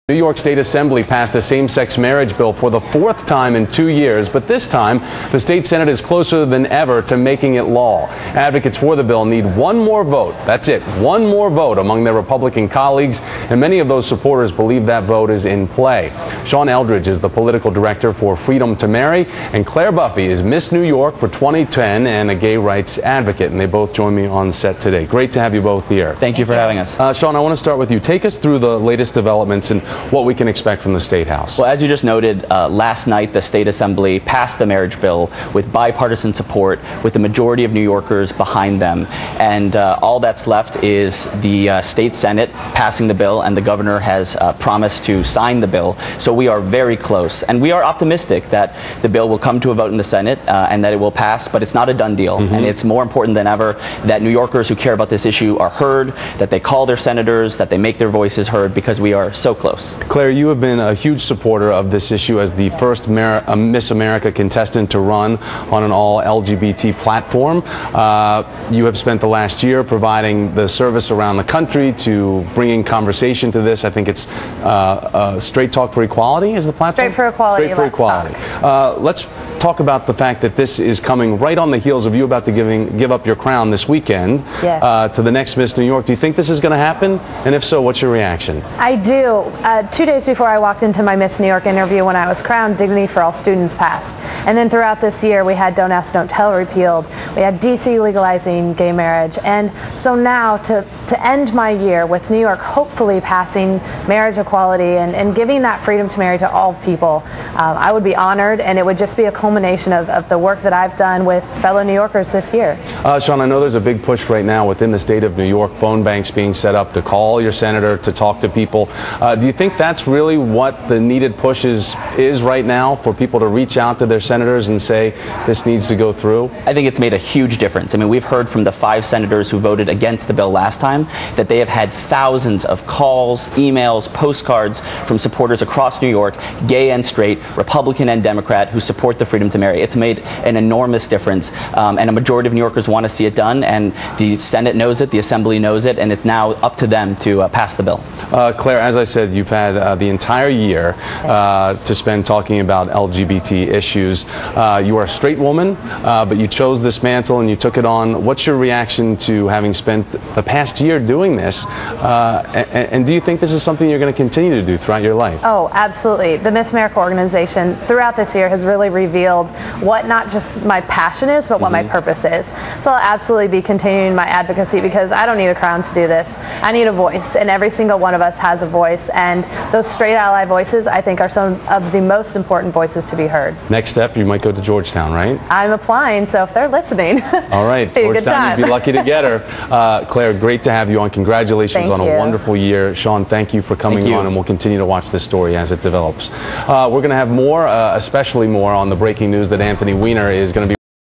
Sean Eldridge and Claire Buffie Discuss NY Marriage Bill with Thomas Roberts, MSNBC
Sean Eldridge, Political Director of Freedom to Marry, and Claire Buffie discuss the New York marriage bill with Thomas Roberts, MSNBC. June 16, 2011